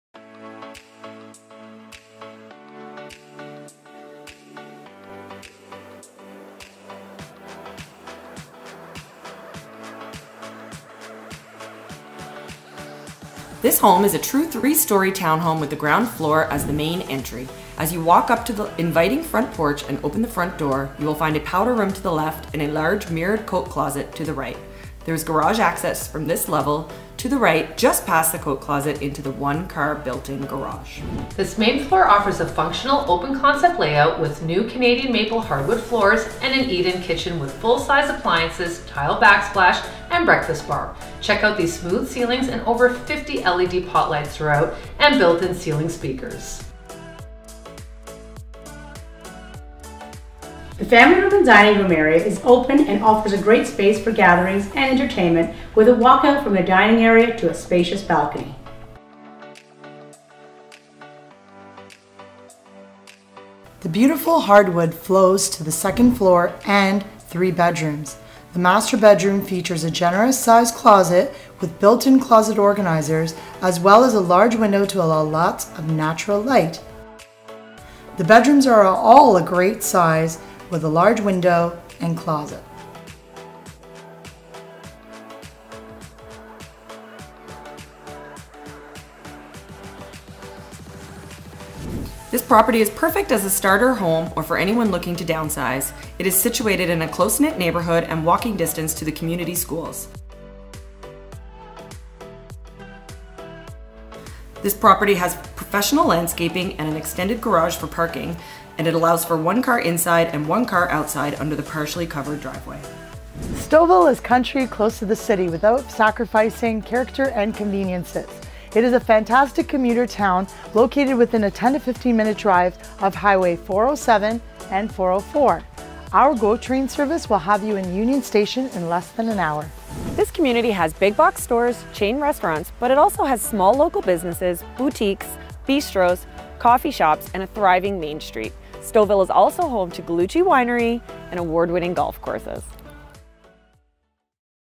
39 Miltrose Crescent, Stouffville Presented By The Trentadue Torres Group
39 Miltrose Crescent Stouffville. This Rare 3 Bedroom Mattamy Built Freehold Townhome Is A Stylish 1124 Sq Ft, End Unit , Bright South Facing With A Walkout To Balcony. Modern Upgrades Like Neutral Paints, Built In Speakers, Nest Thermostat , Cdn Maple Hardwood Throughout, Piano Stained Stairs, Led Potlights On All Levels. Garage Entry To Home. Situated In A Desired School Zone And Close To Smart Centre, Goodlife, Longos Plaza, Starbucks & Main St. Minutes To 404, 407 & Go Station.